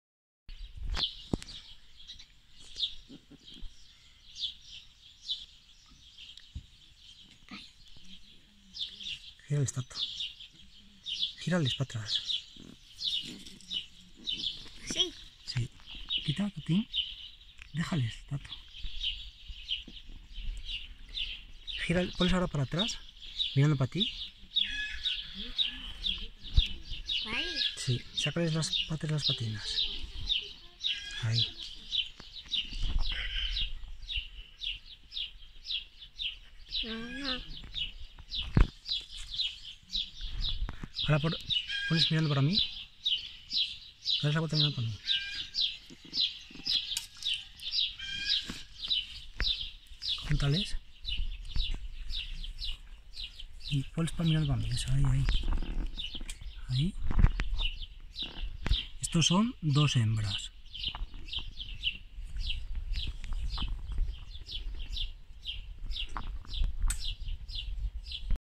Cocker spaniel ingles (hembras) 06-07-20
Cachorros cocker spaniel ingles color dorado sólido con PEDIGREE y línea de sangre de campeones del mundo en su categoría.
Afijo y núcleo zoológico: ENTREVARALES